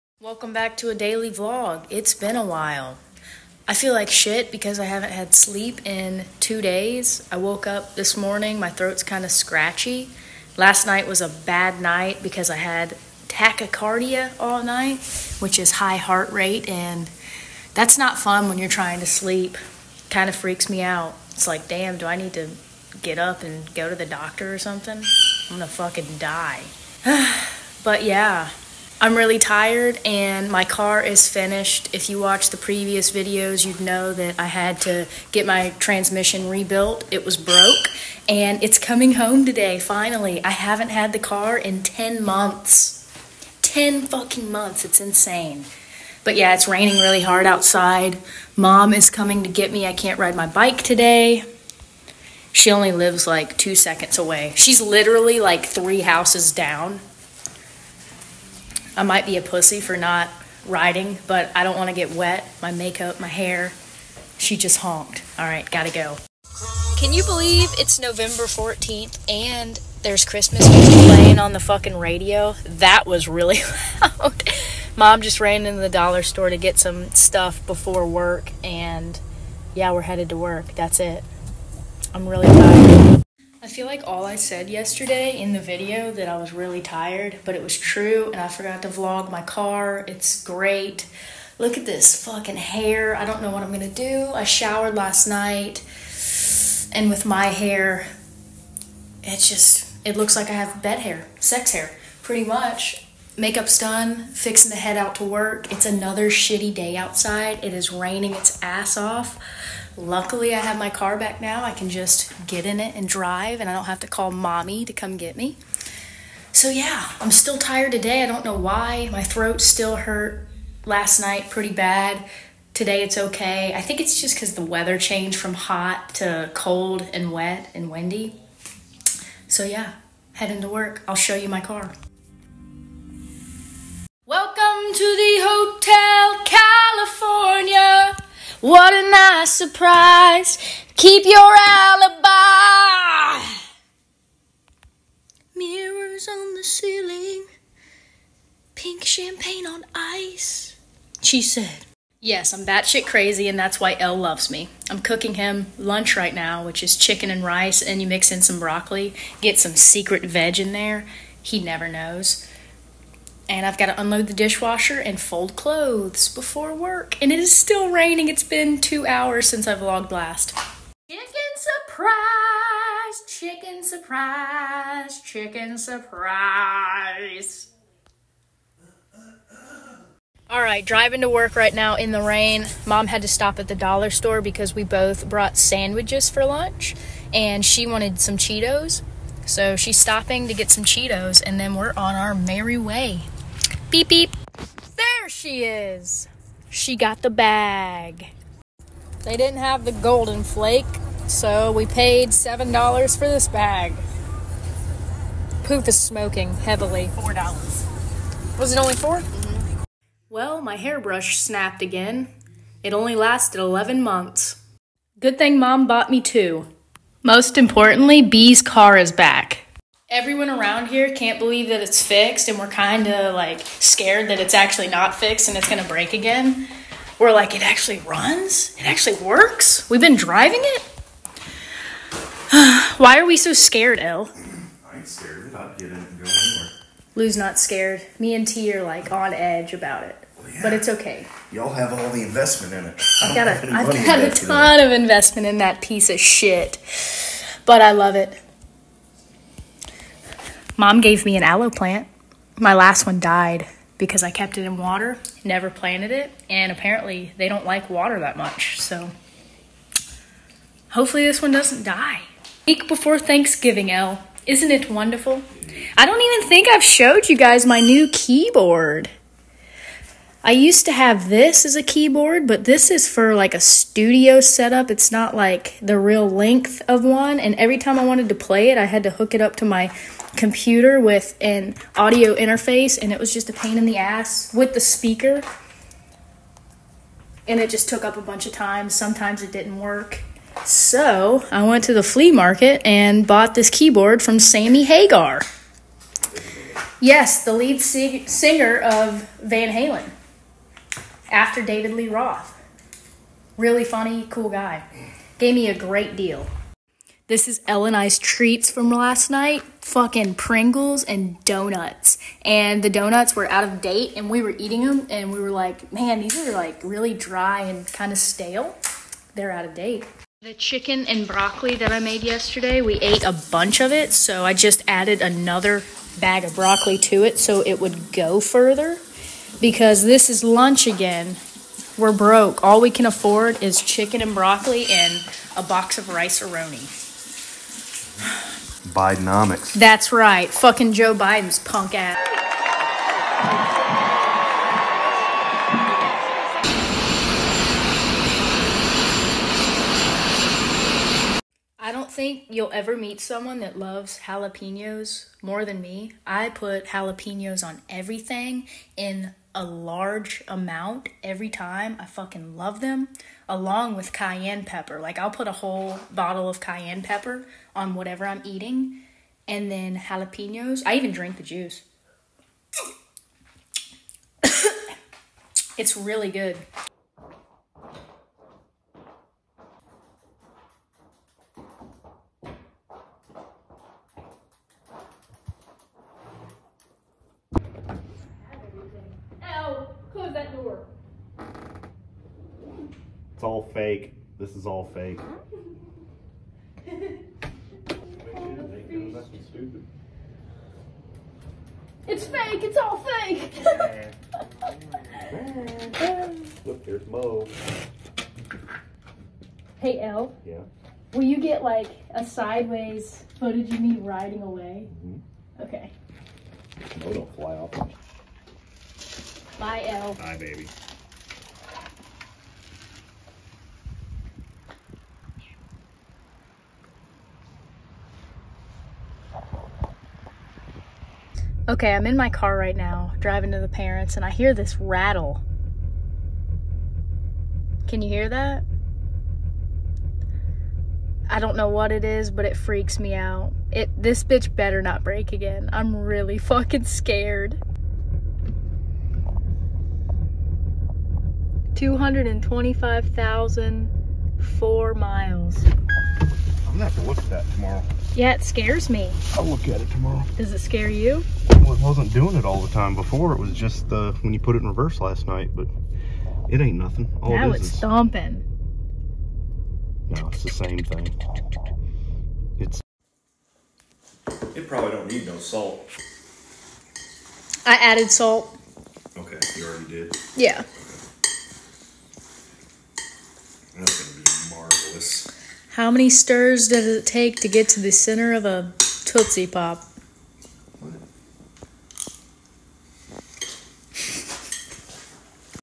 Day In my Life - REAL FOOTAGE - I got my car back- The week before Thanksgiving 2023
Welcome to a non edited Vlog!
I finally Got my Car back!